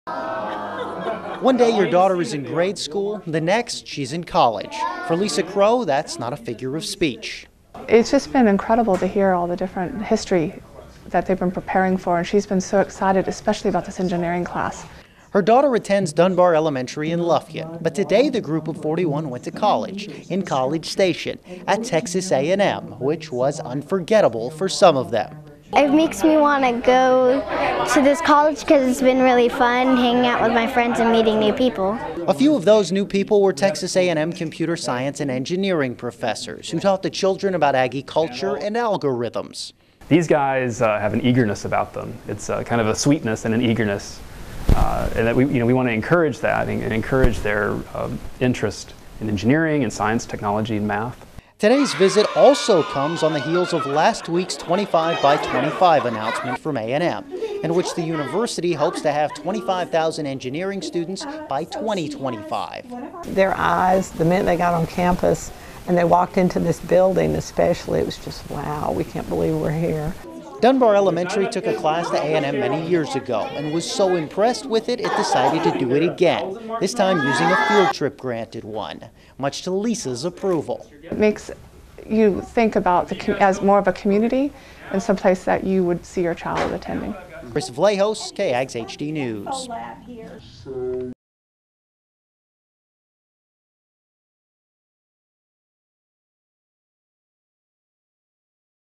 Texas A&M 25 by 25
Last week we told you about Texas A&M's new "25 by 25" initiative to increase the enrollment of engineering students.
    Today, some University Professor's in Computer Science and Engineering started started some students young, in hopes they'll be back.
     KAGS HD News Reporter Chris Vlahos shows what it means for the kids.